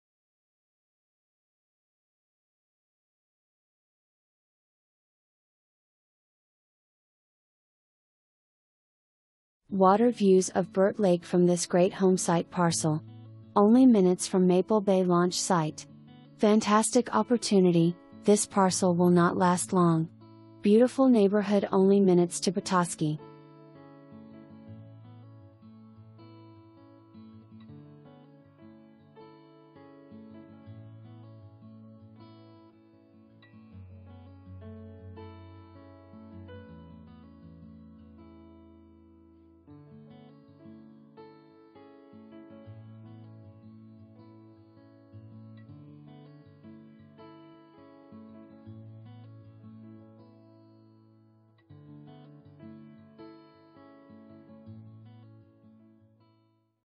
Homes for Sale - 0 Chickagami Trail, Brutus, MI
Water views of Burt Lake from this great homesite parcel. Only minutes from Maple Bay launch site. Fantastic opportunity, this parcel will not last long. Beautiful neighborhood only minutes to Petoskey.

Karen Petrimoulx
Coldwell Banker Fairbairn